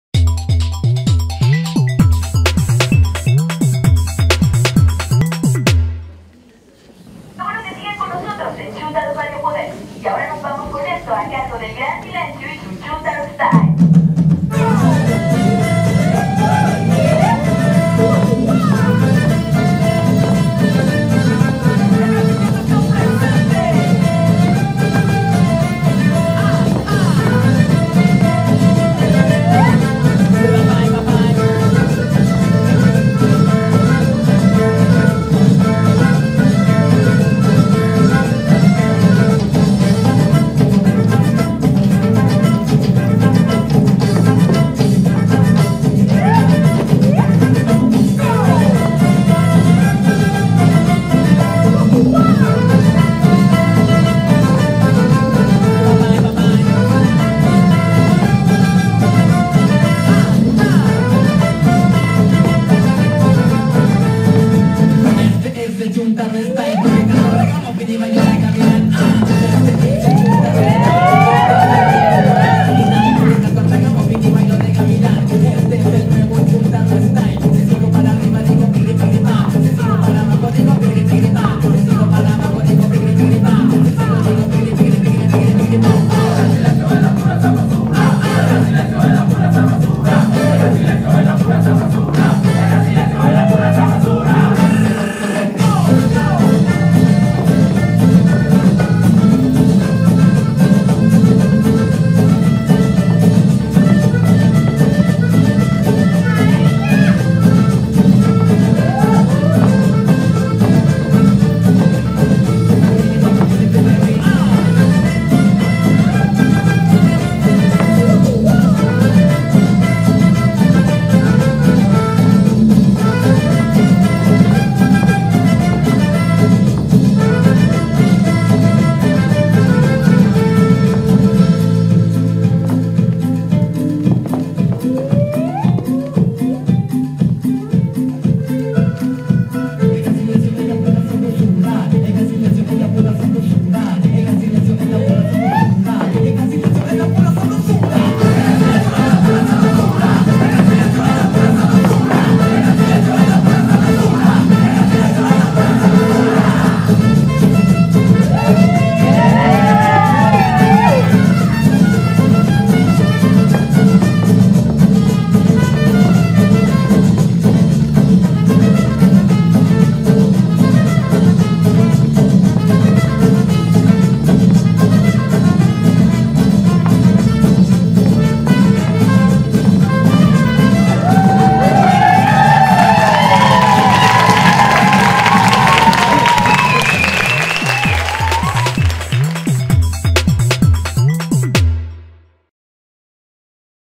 Fusion Freeze II Show: Chimella
Performance of Chimella @ the show of Fusion Freeze II, a tribal and fusion bellydance festival hosted by Teuta.
Location: Planeet Mars, Gent, Belgium